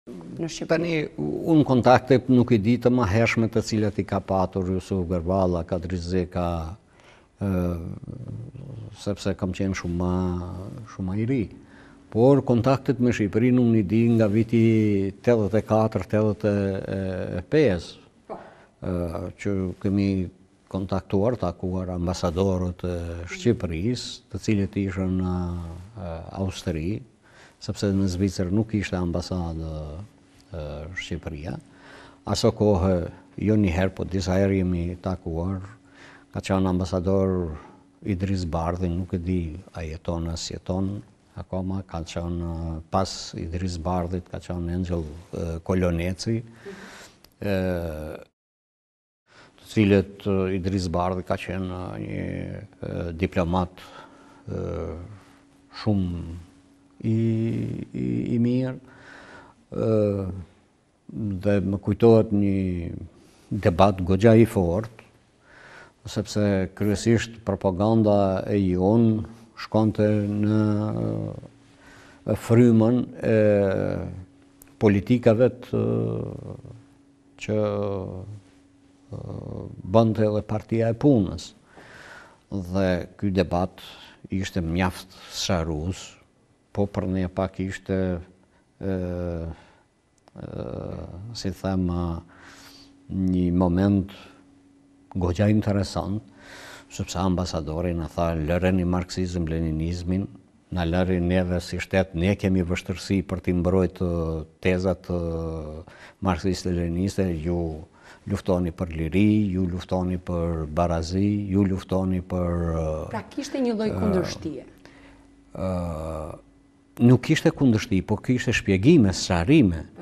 “Lereni Marksizëm-Leninizmin…”/ Ali Ahmeti rrëfen “porosinë” e ambasadorit shqiptar në Austri
Cilat ishin kontaktet e lëvizjeve ilegale në Kosovë me regjimin komunist në Shqipëri?

Kësaj pyetjeje i ka dhënë përgjigje kreu i BDI, Ali Ahmeti në një intervistë ekskluzive për Ora News.

Sipas tij, kontaktet me Shqipërinë ka qenë rreth viteve 84-85, ndërsa rrëfehu se çfarë u kishte thënë ambasadori i Shqipërisë në Austri ato vite, Idriz Bardhi.

“Unë kontaktet me Shqipërinë i di nga vitet 84, 85. Kemi kontaktuar, takuar ambasadorët e Shqipërisë, të cilët ishin në Austri sepse në Zvicër nuk kishte ambasadë. Aso kohe , jo një herë por disa herë jemi takuar. Ka qenë ambasador Idriz Bardhi, nuk e di a jeton a nuk jeton. Pas Idriz Bardhit ka qenë Engjëll Koloneci. Idriz Bardhi ka qenë një diplomat shumë i mirë dhe më kujtohet një debat goxha i fortë sepse kryesisht propaganda jonë shkonte në frymën e politikave që bënte dhe Partia e Punës. Ky debat ishte mjaft sqarues. Lerini Marksizëm-Leninizmit. Edhe ne si shtet kemi vështirësi ti mbrojmë këto teza. Ju luftoni për liri. Ju luftoni për barazi. Nuk ishte kundërshti por sqarime.